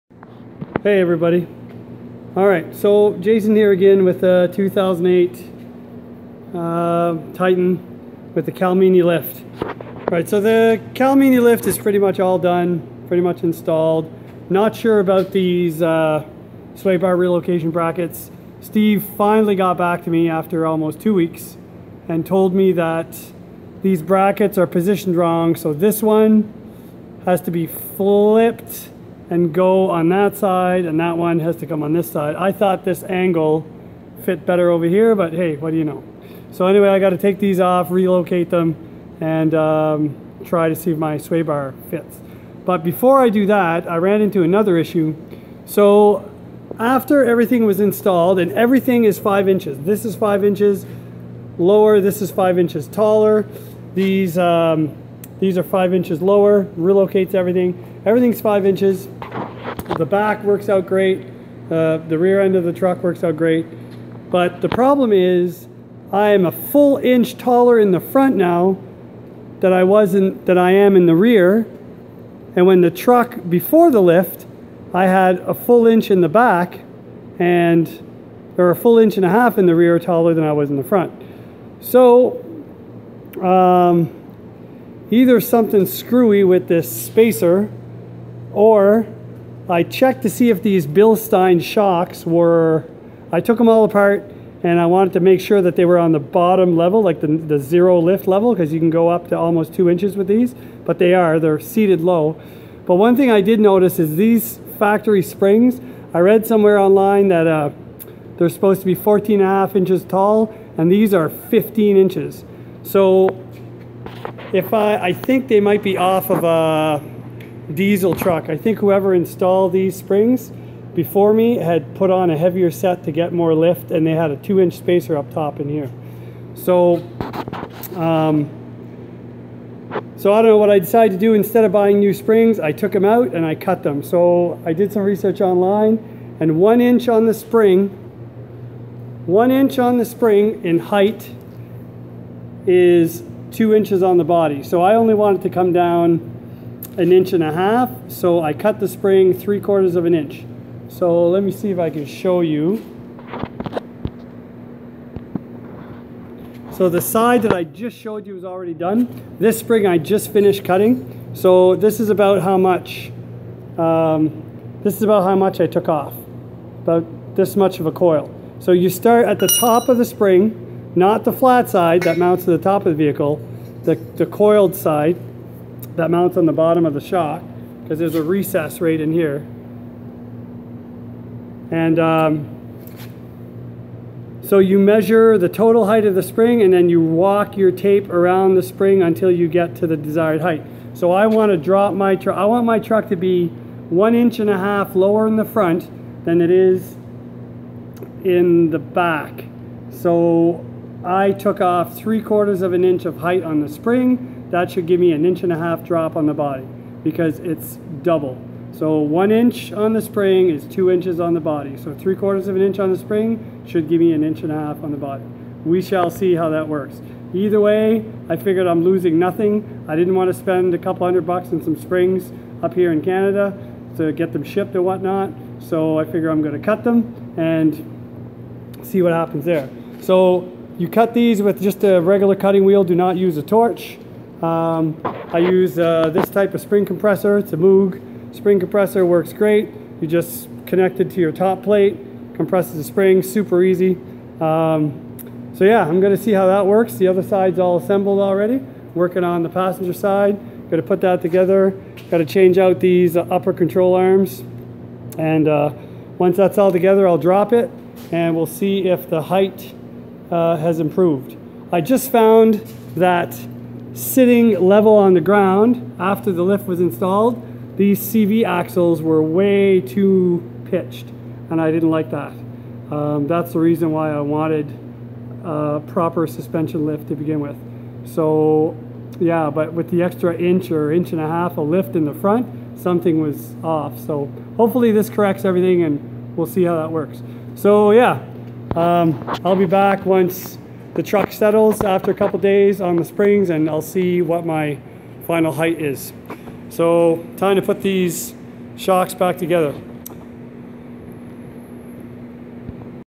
Calmini 6 inch Titan lift #16: front is sitting higher than the rear!
After the lift, the front was sitting a full inch higher than the rear. prior to the lift, the front was an inch lower. In this video, I'm gonna cut 3/4 of an inch off of  the springs which translates to 1.5 inches lower on the body, and should give me 1/2 and inch lower in the front than the rear. we'll see!